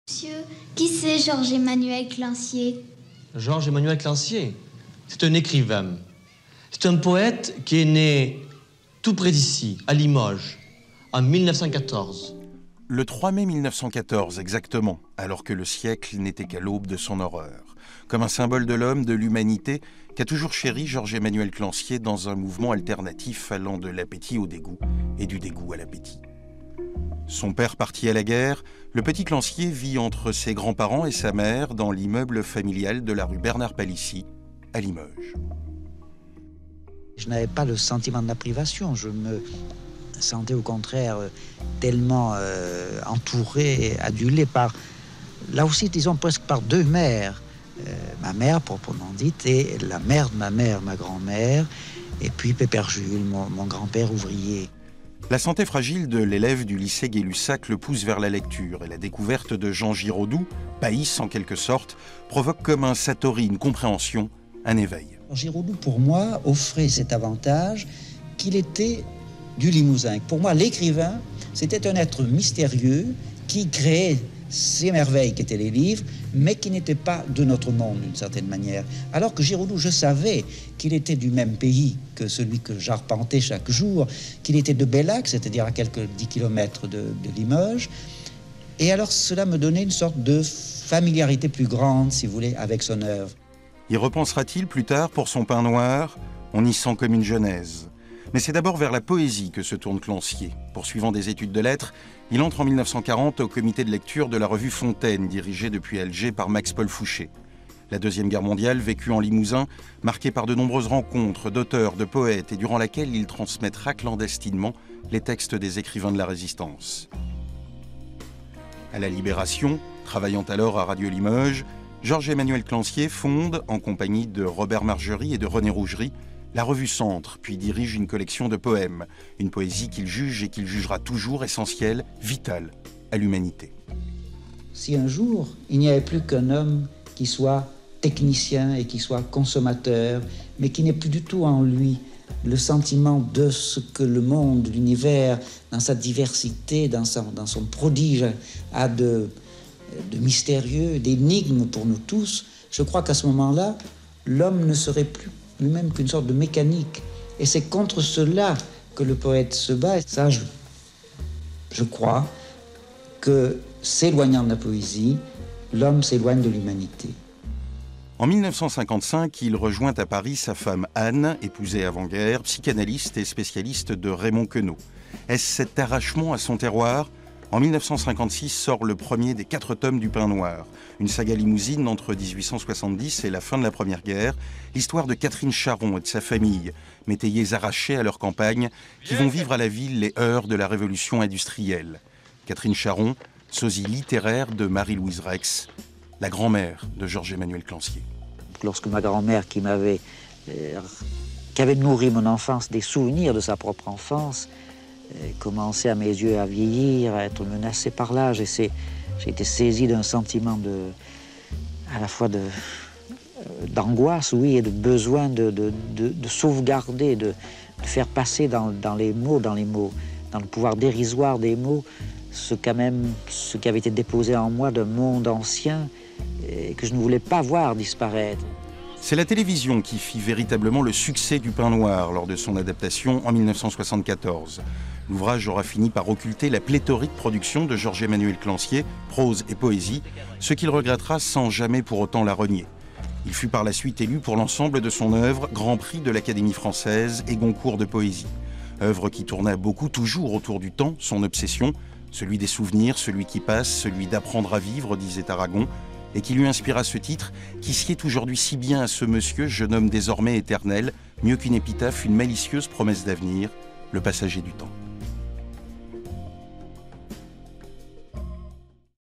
Georges-Emmanuel Clancier, l'écrivain du Limousin
Georges-Emmanuel Clancier, l'écrivain du Limousin est décédé le 4 juillet 2018 à l'âge de 104 ans.
Retour sur sa vie et son oeuvre avec Jean-Martial Jonquard.